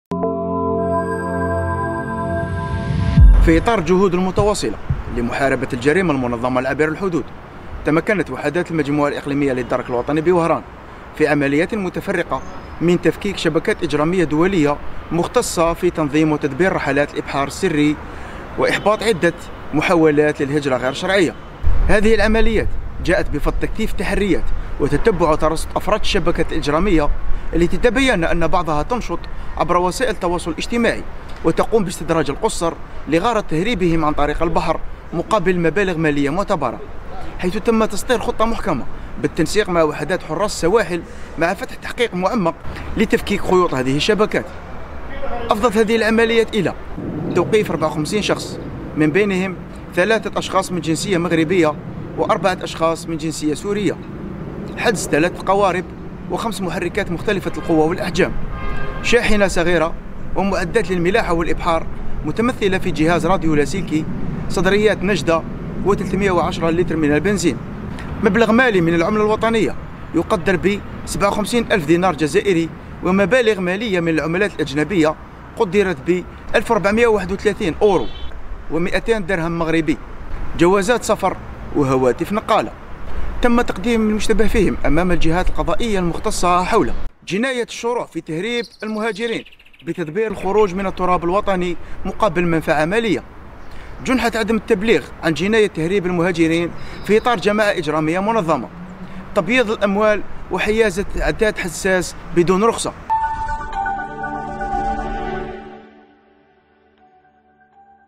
وهران: توقيف 54 شخصا اثر تفكيك شبكات إجرامية دولية مختصة في تنظيم وتدبير رحلات الإبحار السري
الجزائر(وأج)- تمكنت وحدات المجموعة الإقليمية للدرك الوطني بوهران،  من توقيف 54 شخصا في عمليات متفرقة، اثر تفكيك شبكات إجرامية دولية مختصة في تنظيم وتدبير رحلات الإبحار السري، حسب ما أورده بيان لقيادة الدرك الوطني.